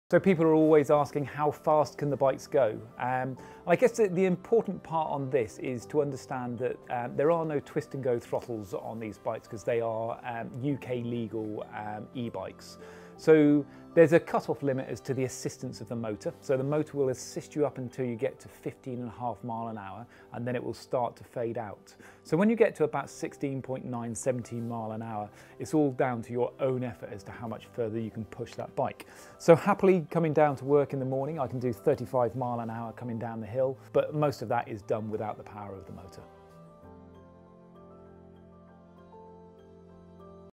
How fast can a Riese & Müller e-bike go?
EDEMO's Dan takes you through the answer to another of our customers' frequently asked questions about Riese & Müller's electric bike range. To get in touch, head on over or give us a call on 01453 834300.